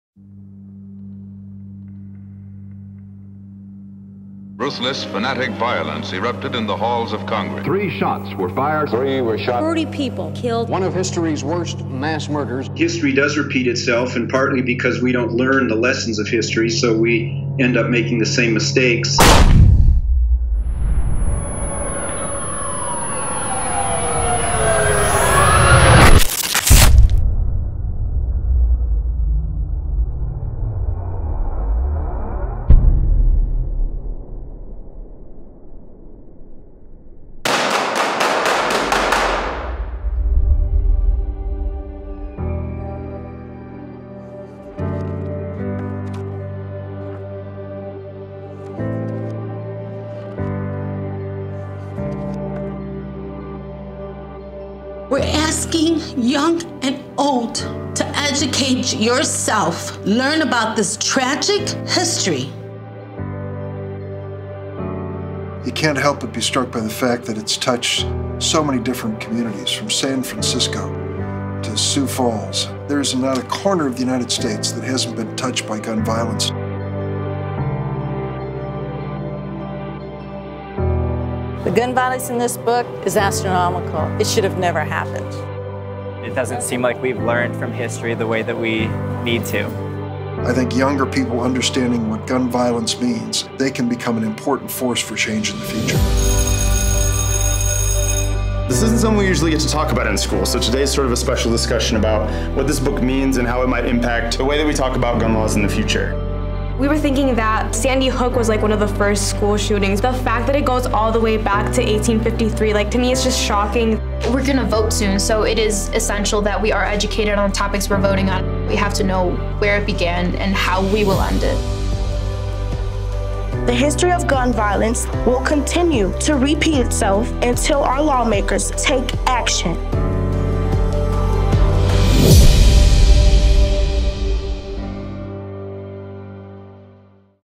The Gun Violence History Book – Social Film
Introducing the Gun Violence History Book. A powerful symbol and an education tool. Help us to make these books available to schools across America at stopgunviolencehistory.com. Together, we can stop history from repeating itself.